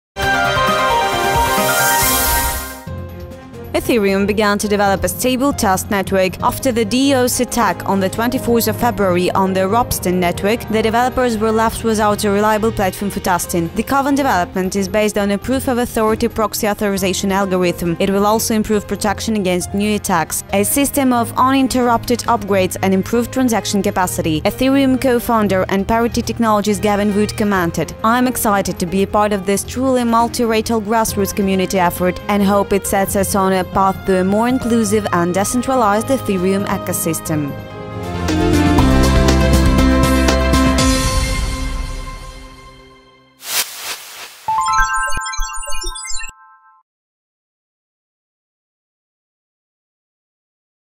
KCN Ethereum launches new test network
Ethereum began to develop a stable test network. After the DoS attack on 24 of February on the Ropsten network, the developers were left without a reliable platform for testing. The Kovan development is based on the Proof of Authority proxy authorization algorithm, it will also improve protection against new attacks, a system of uninterrupted upgrades and improved transaction capacity. Ethereum co-founder and Parity Technologies Gavin Wood commented: "I'm excited to be a part of this truly multilateral grassroots community effort and hope it sets us on a path to a more inclusive and decentralised Ethereum ecosystem".